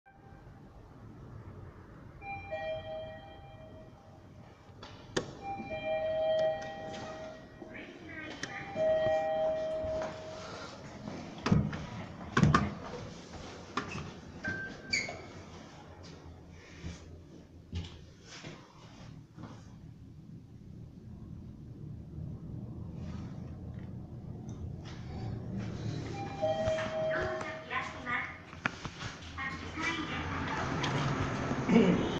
三菱エレベーター
シンボルタワー高松にあります。